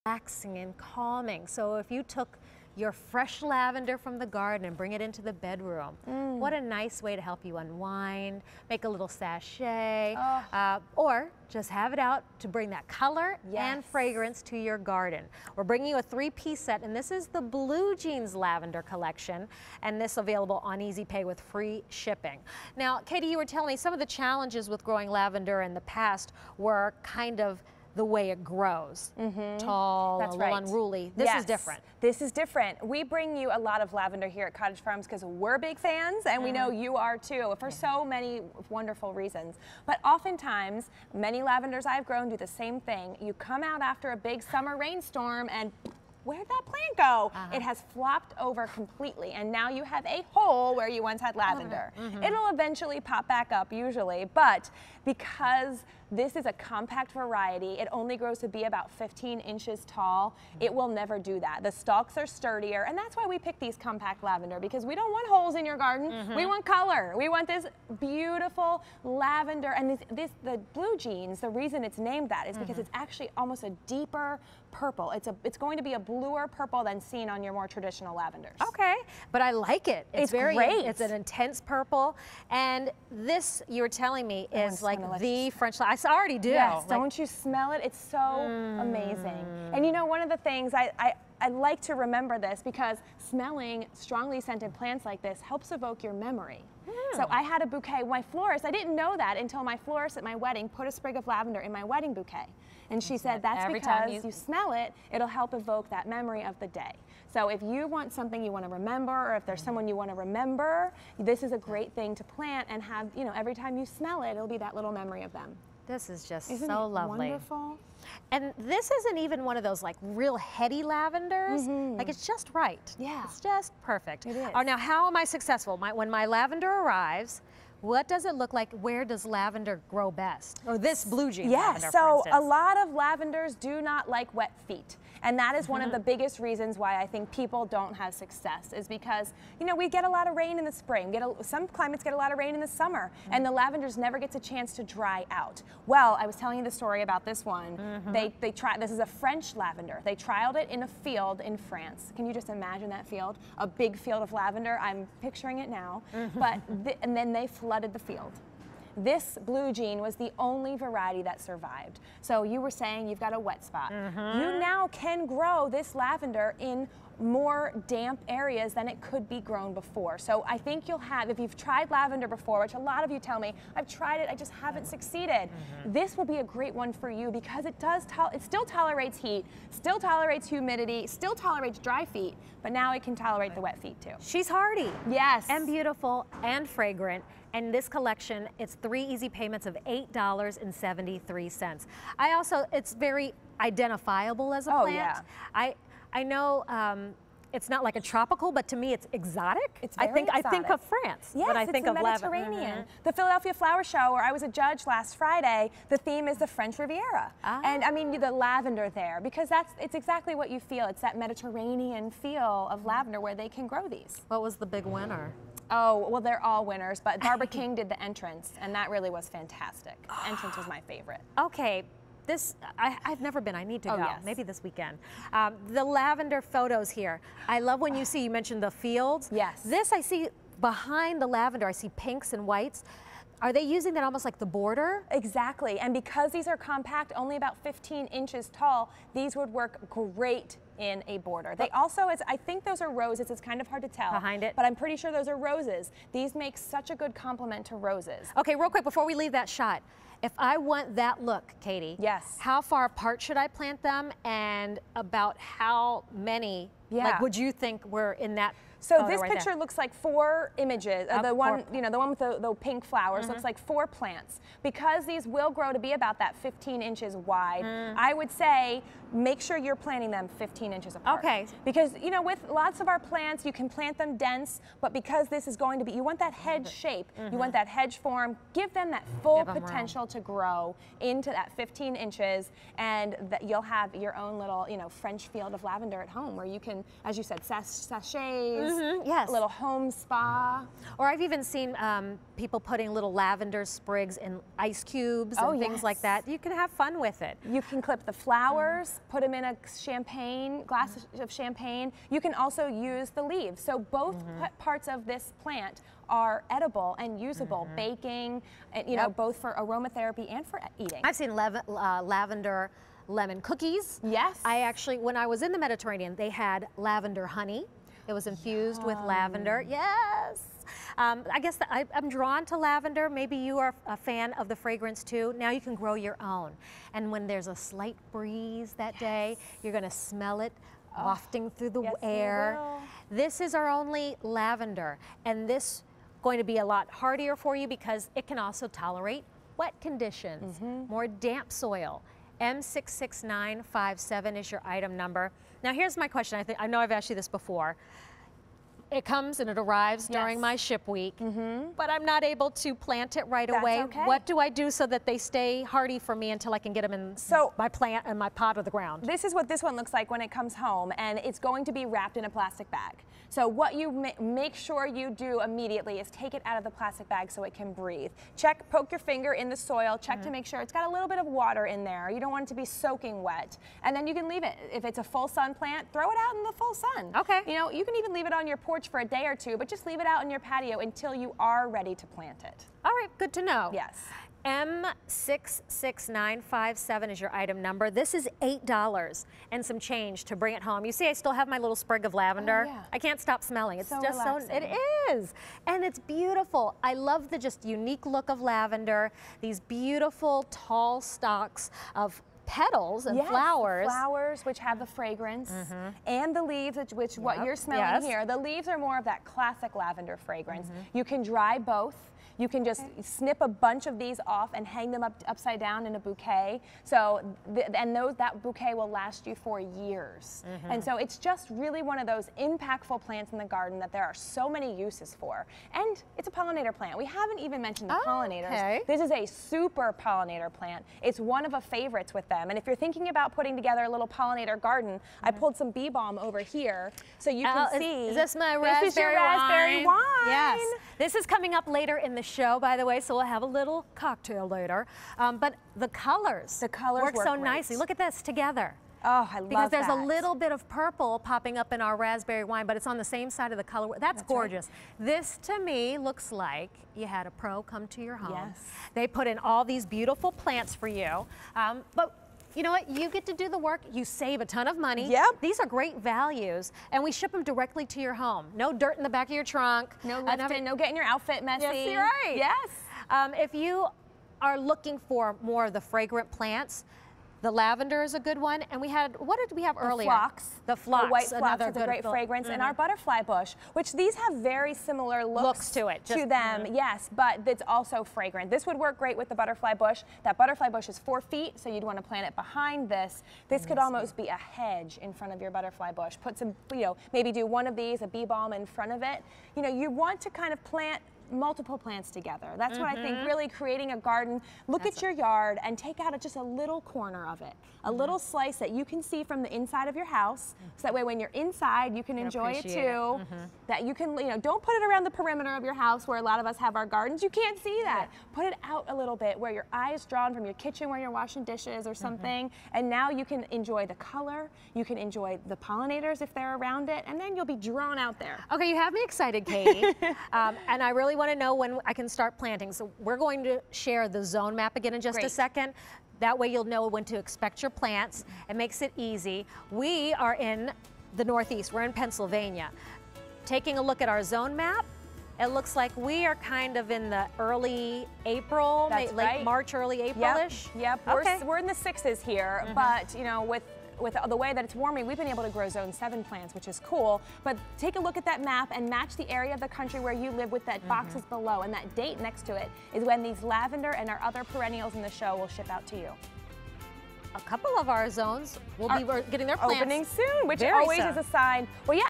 Cottage Farms 3-Piece Blue Jeans Lavender on QVC
Cottage Farms 3-Piece Blue Jeans Lavender

As classic as a pair of your favorite blue jeans, these lavender plants add beauty and a familiar fragrance to gardens, fresh arrangements, and dried flower projects. From Cottage Farms.